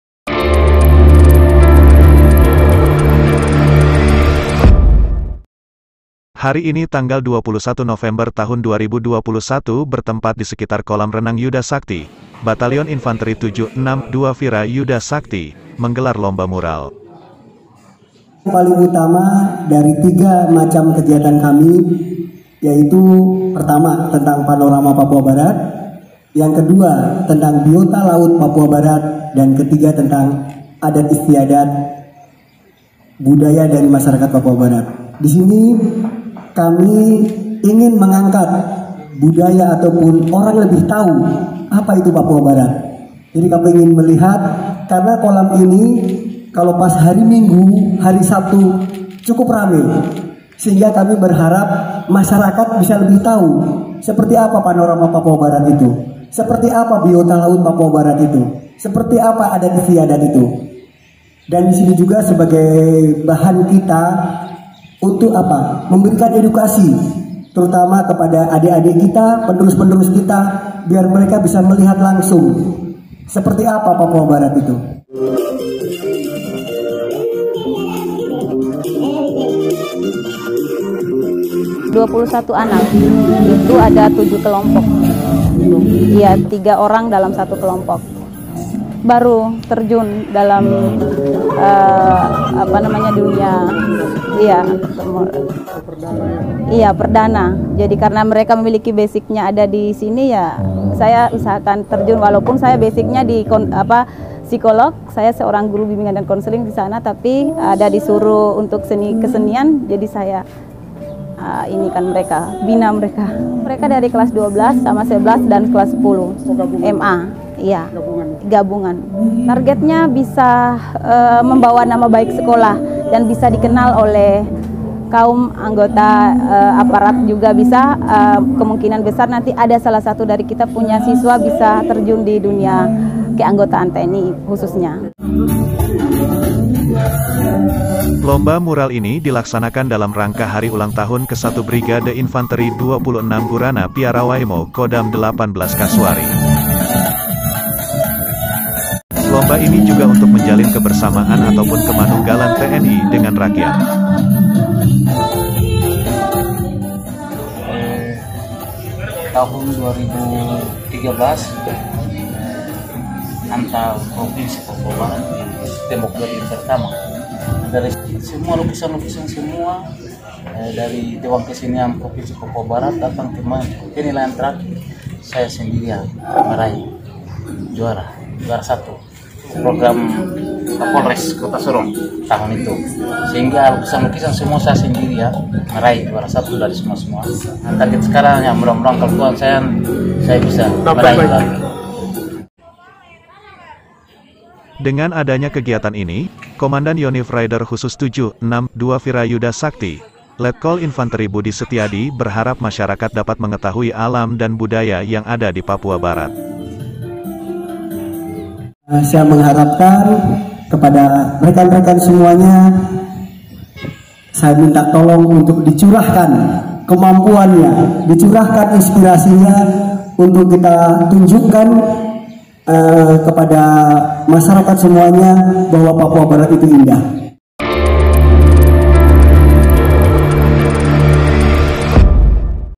Yonif Raider Khusus 762/Vira Yudha Sakti Gelar Lomba Mural
Lomba Mural ini dilaksanakan dalam rangka Hari Ulang Tahun ke 1 Brigade Infanteri 26/Gurana Piarawaimo Kodam XVIII/Kasuari.

Lomba Mural ini dibuka Danyonif RK 762/VYS Letkol Inf Budi Setiadi.